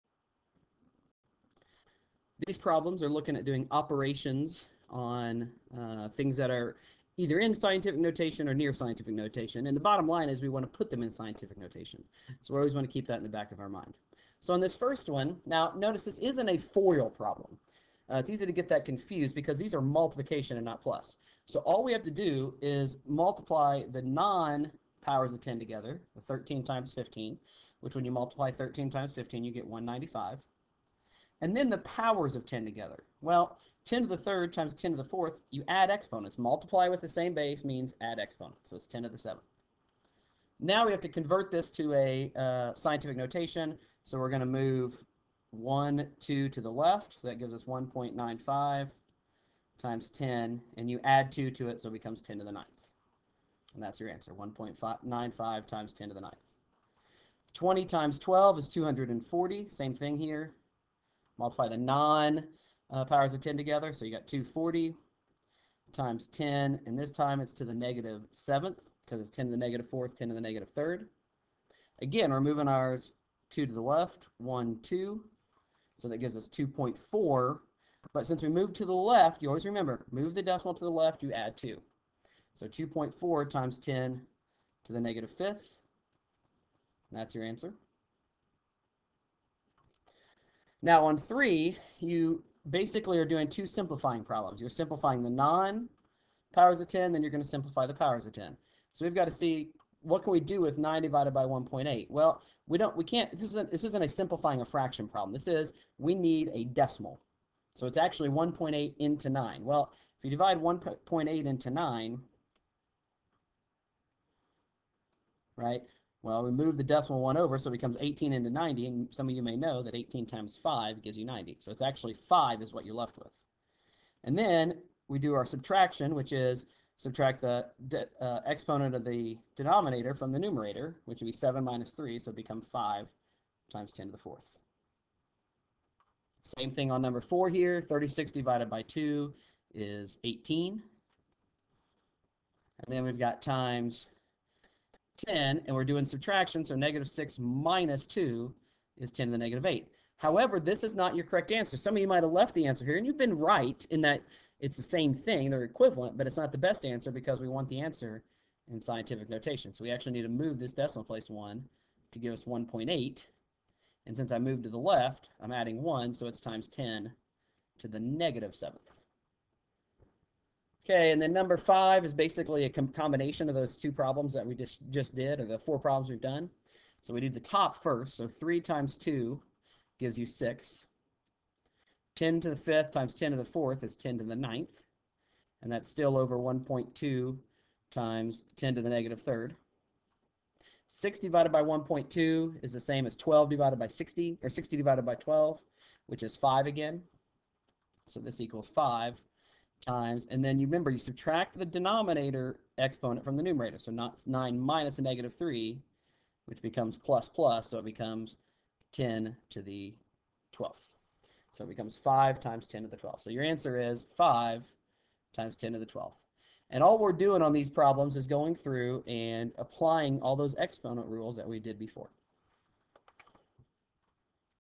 Scientific Notation Part 2
Five problems showing how to multiply and divide expressions written in scientfic notation.  For organized video math lessons check out